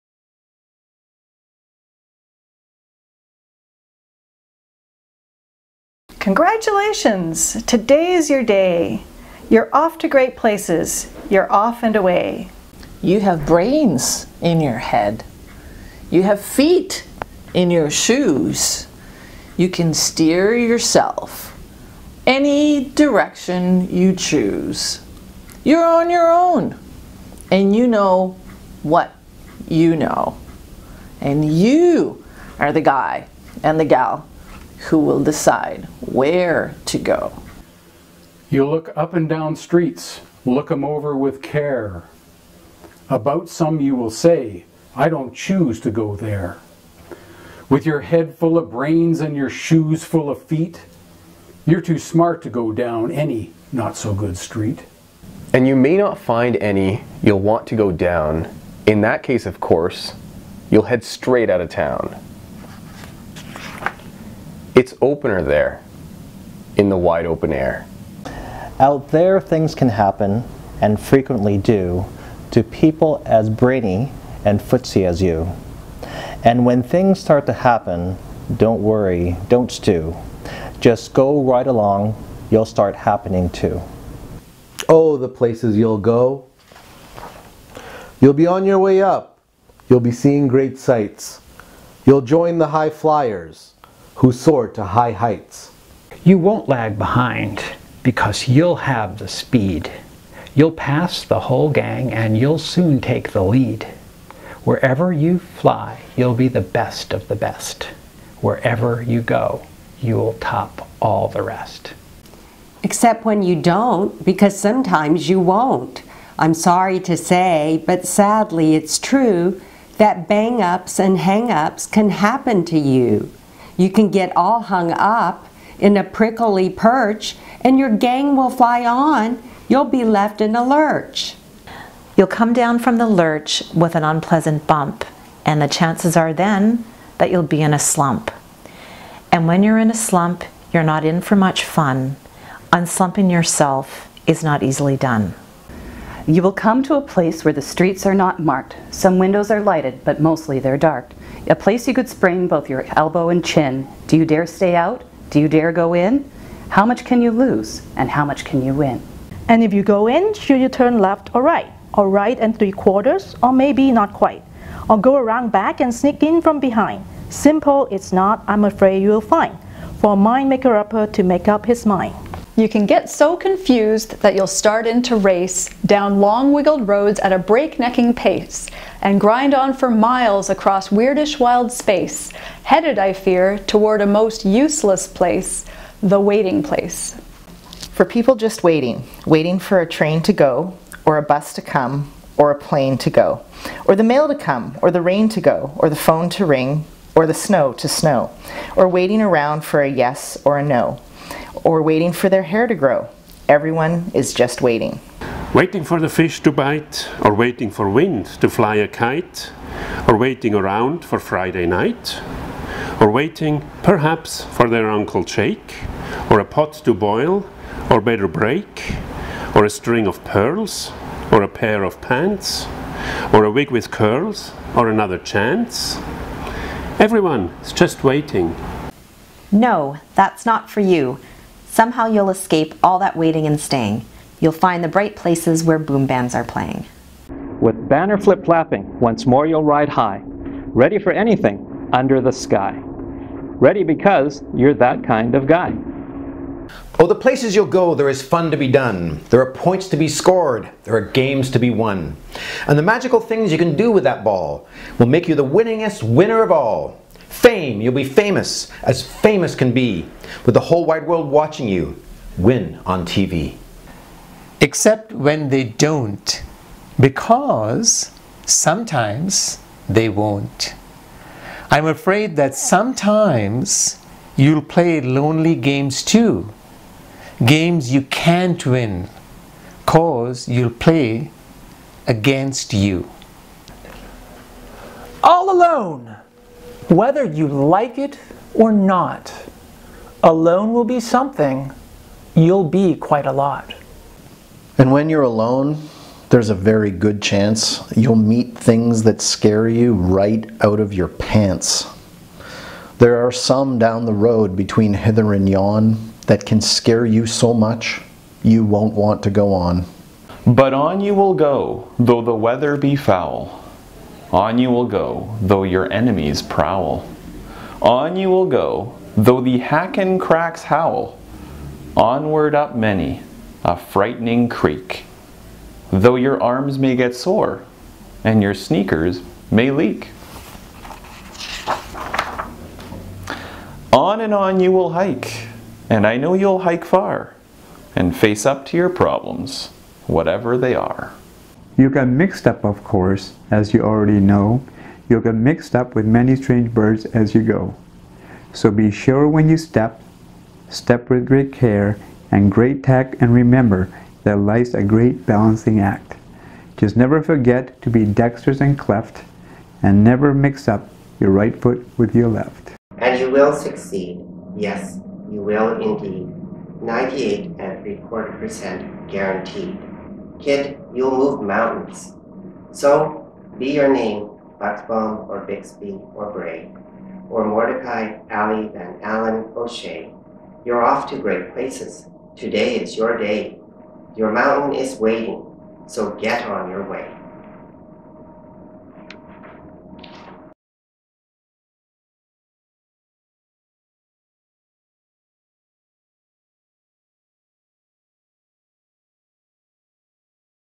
Oh, the Places You'll Go! By UBC Pharmacy Instructors
To the UBC Pharmacy instructors: a HUGE heartfelt thank you goes out to each and every one of you; without you this video would not be possible

To my class, the grad class of 2017: I wish you all the best, and hope this video will help inspire you for your future endeavors, whatever they may be

Special thank you to Adam Smylie for the original idea.

This has been a low-budge, amateur-produced video. Hope you enjoyed watching it!